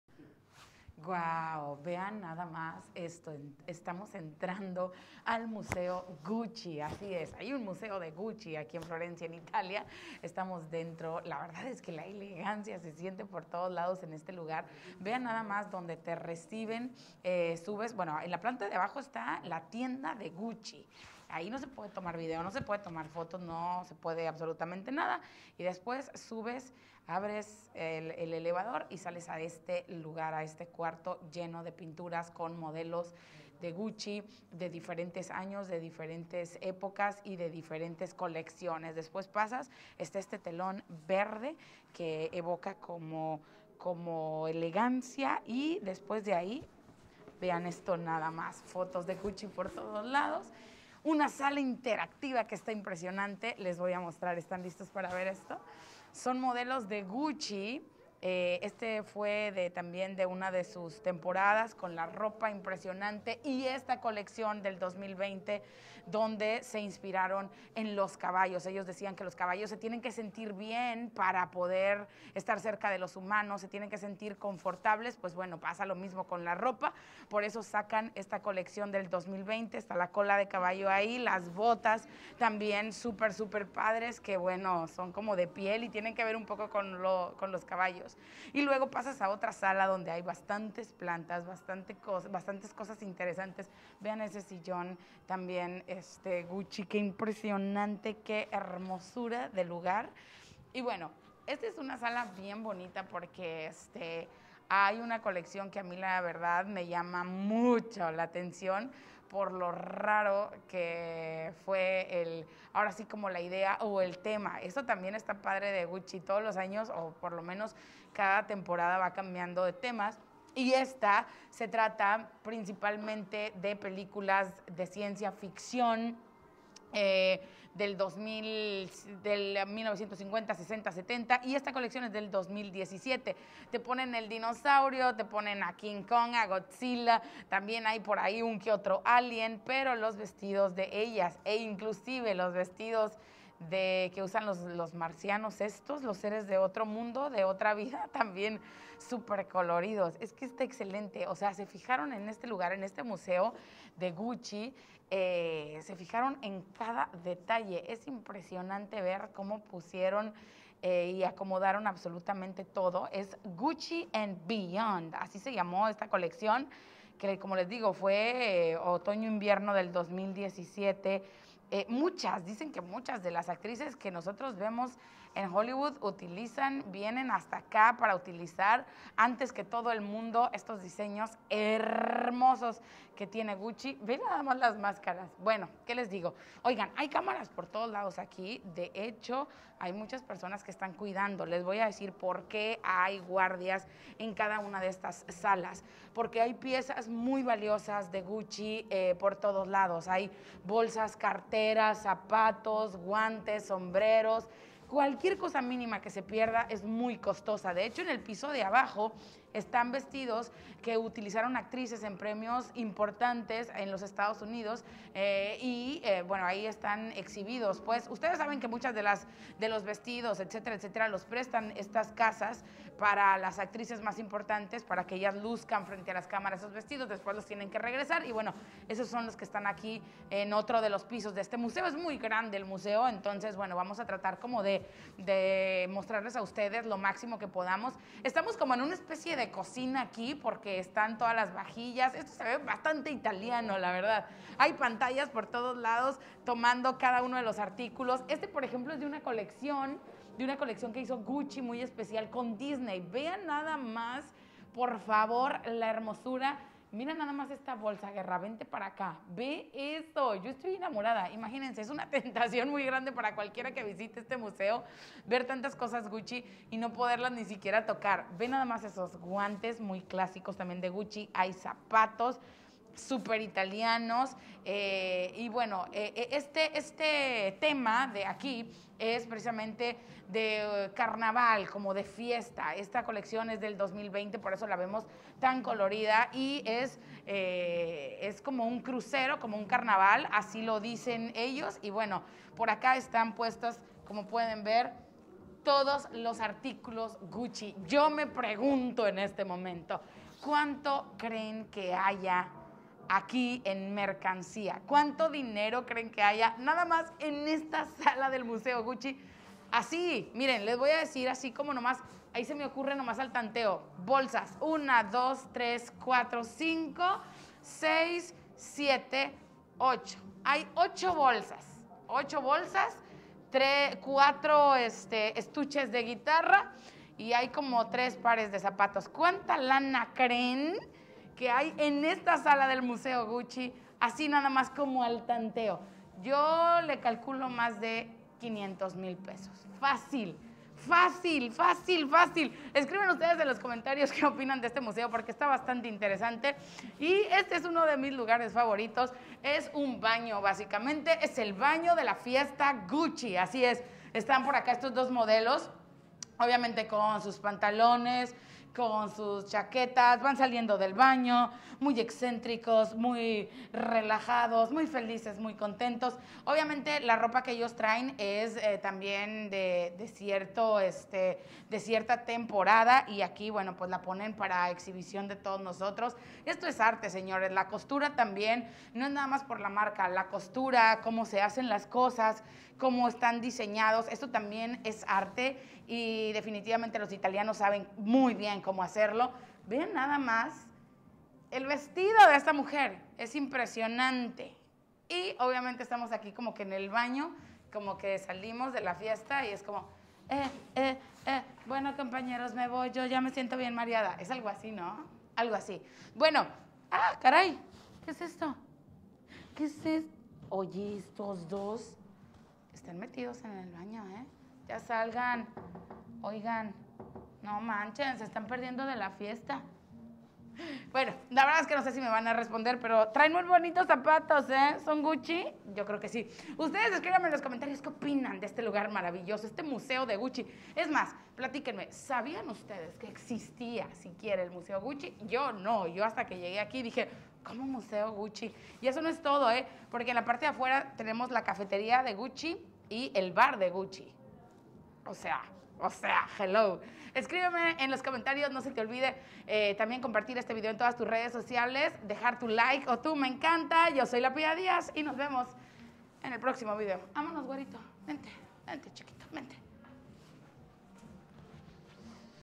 Así son los museos de los millonarios
En Florencia, Italia existe un Museo Gucci, este es dedicado a la emblemática marca de moda con exposiciones de prendas y bolsos clásicos. Nuestra @LaPiaDiaz nos enseña un poquito de lo que encontrarán en este museo. ¿Qué marca de moda les gusta?

BADABUN NETWORK ©